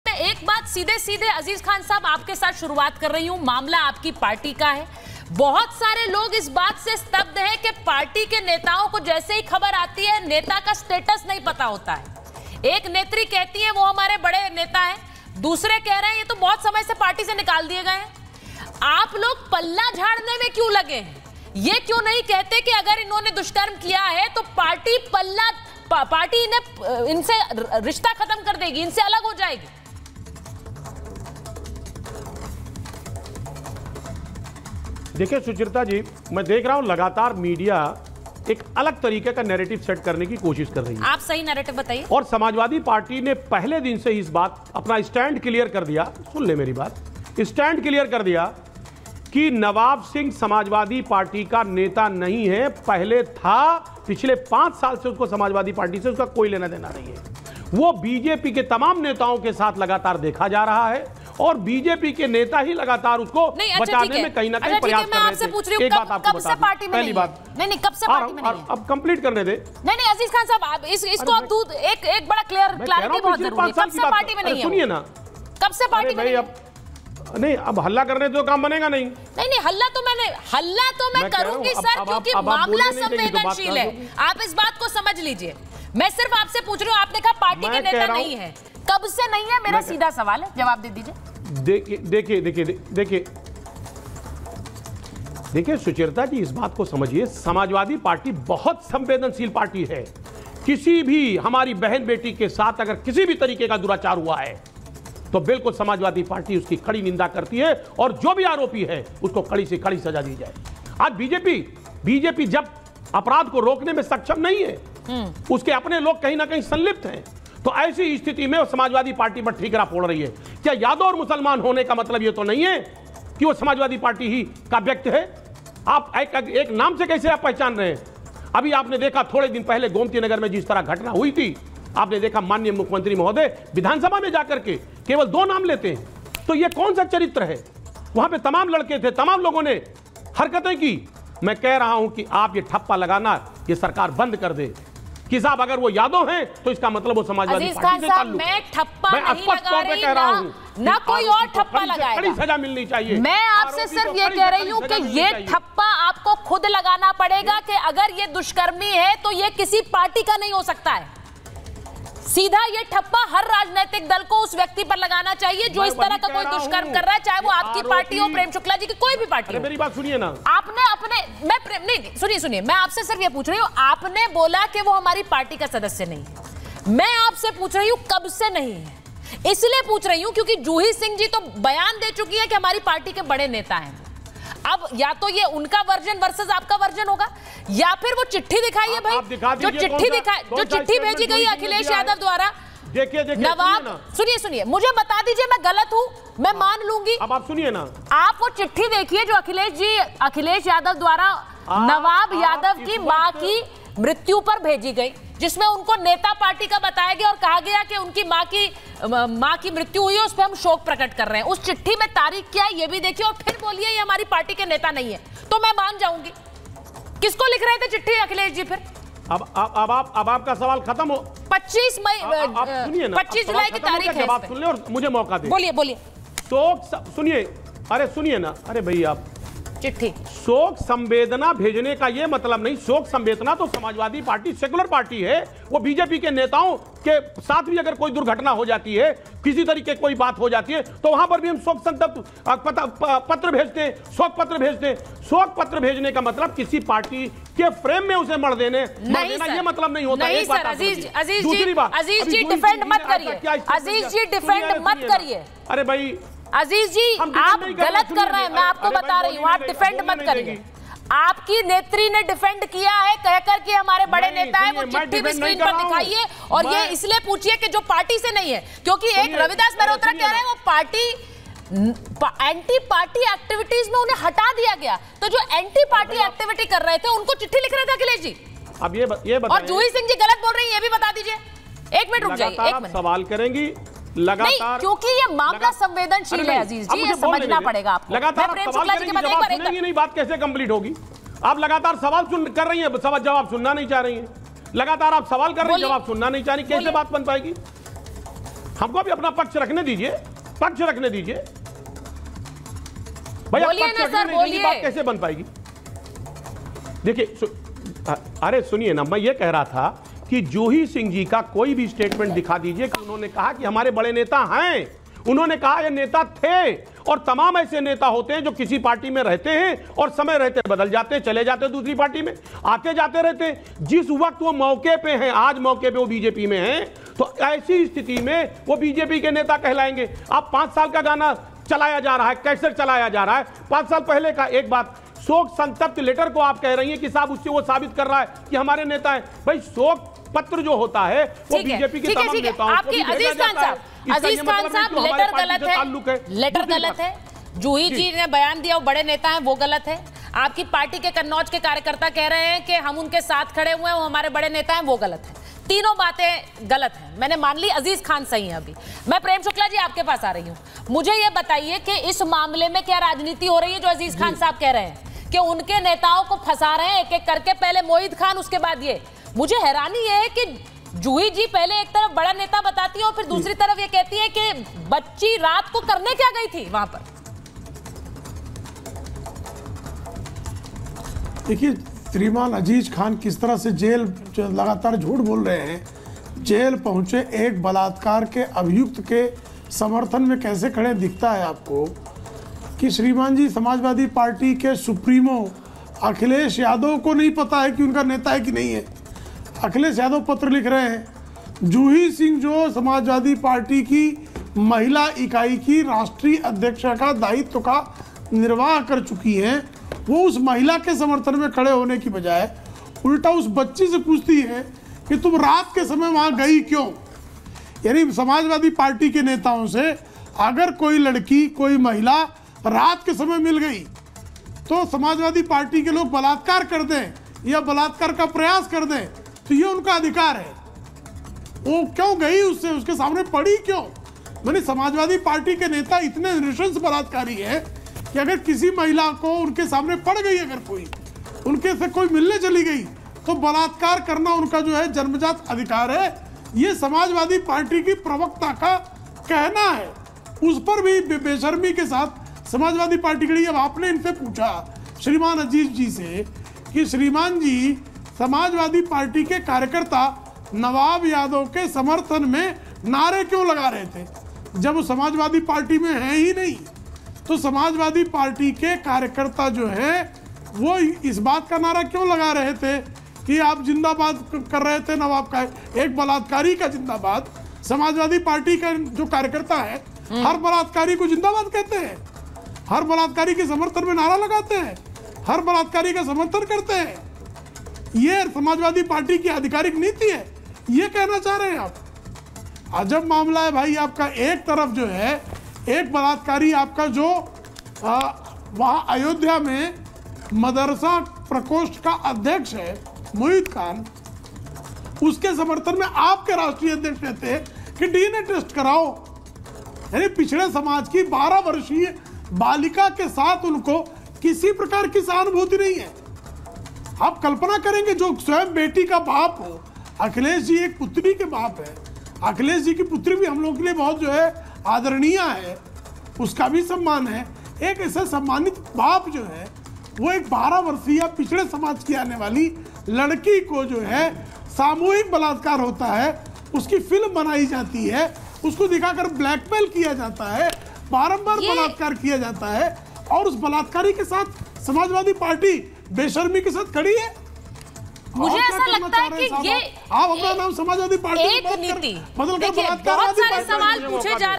Aziz Khan का बड़ा बयान, Nawab Singh को बता दिया BJP का साथी ! | R Bharat
उत्तर प्रदेश के कन्नौज में नाबालिग से रेप की कोशिश के आरोप में पुलिस ने एक सपा नेता को गिरफ्तार किया है। आरोपी नेता सपा प्रमुख अखिलेश यादव का करीबी और उनकी पत्नी डिंपल का सांसद प्रतिनिधि रहा है। मामला कन्नौज कोतवाली के अड़ंगापुर गांव का है।